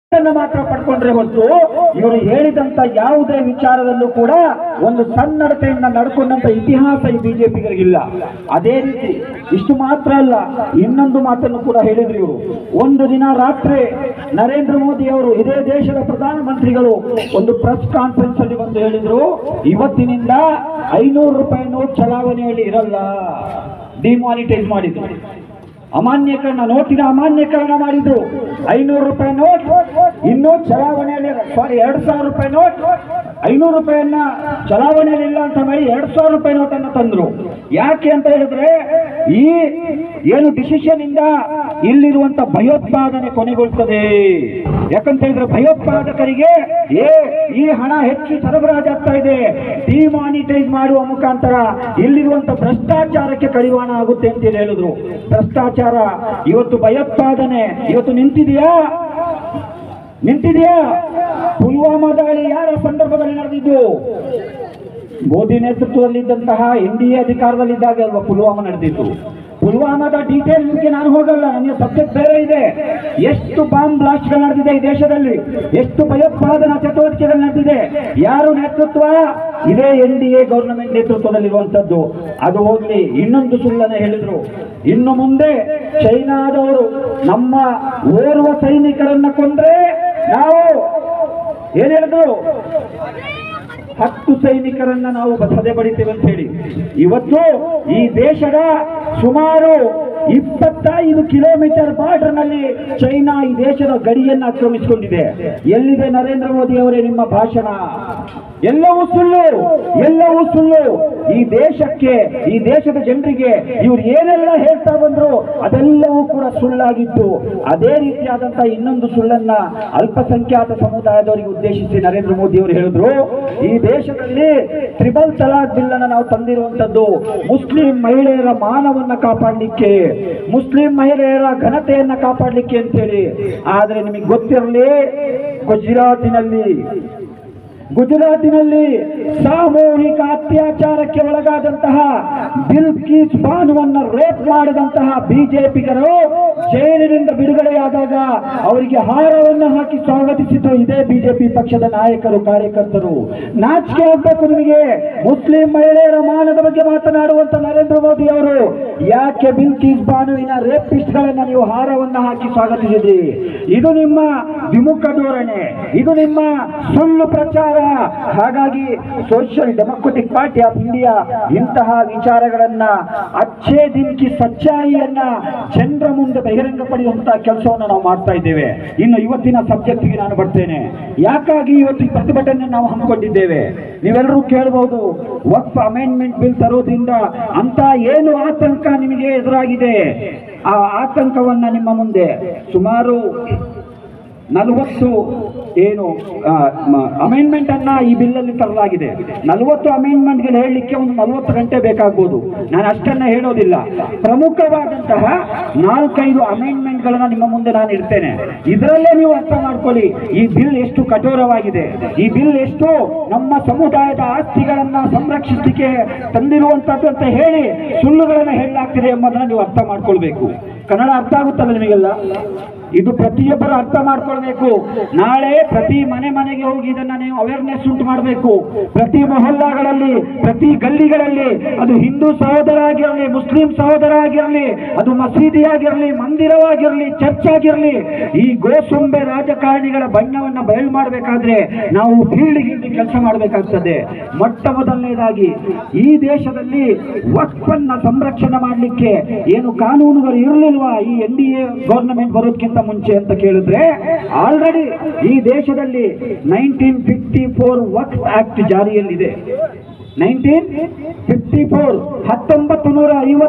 ವಖ್ಫ್‌ ತಿದ್ದುಪಡಿ ಮಸೂದೆ - 2024 ಕಾಯ್ದೆ ಬಗ್ಗೆ ವಿವರಿಸಿದ ರಾಜ್ಯ ಪ್ರಧಾನ ಕಾರ್ಯದರ್ಶಿ ಅಫ್ಸರ್‌ ಕೊಡ್ಲಿಪೇಟೆ
ಶಿಗ್ಗಾಂವಿಯಲ್ಲಿ ನಡೆದ ವಖ್ಫ್‌ ತಿದ್ದುಪಡಿ ಮಸೂದೆ - 2024 ವಿರುದ್ದದ ಪ್ರತಿಭಟನೆಯಲ್ಲಿ ಕಾಯ್ದೆ ಬಗ್ಗೆ ಸಂಪೂರ್ಣವಾಗಿ ವಿವರಿಸಿದ ರಾಜ್ಯ ಪ್ರಧಾನ ಕಾರ್ಯದರ್ಶಿ ಅಫ್ಸರ್‌ ಕೊಡ್ಲಿಪೇಟೆ